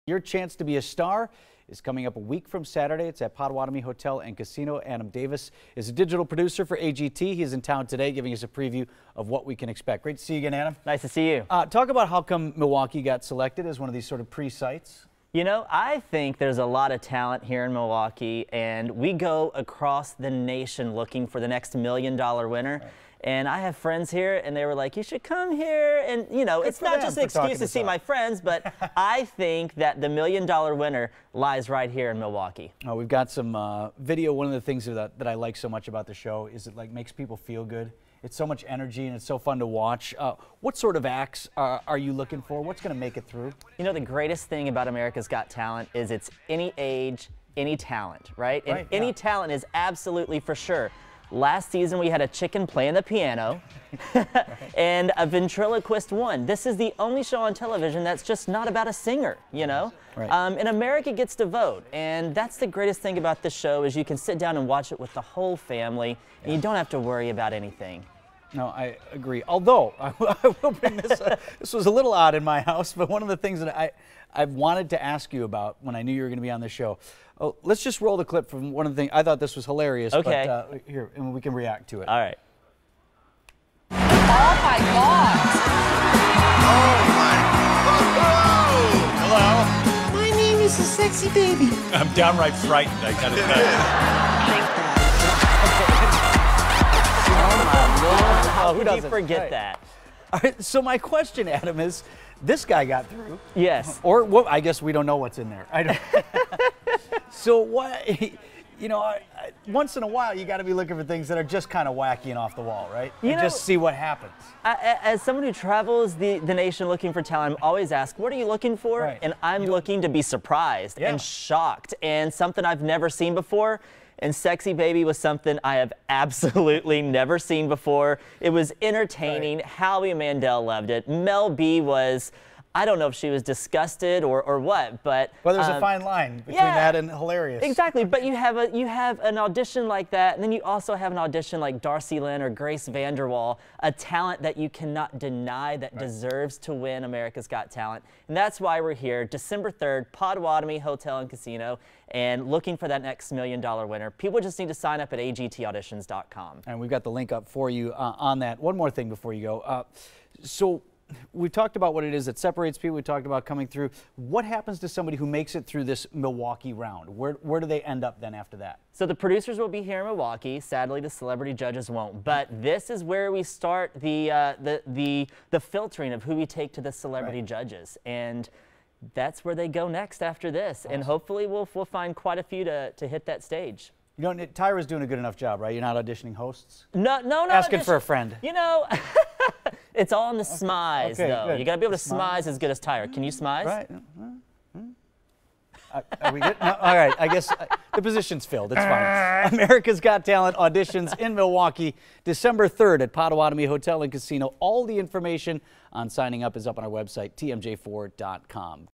'America's Got Talent' holding open auditions in Milwaukee this December
"America’s Got Talent" is holding open auditions for its 13th season on Sunday, December 3 at the Potawatomi Hotel &amp; Casino.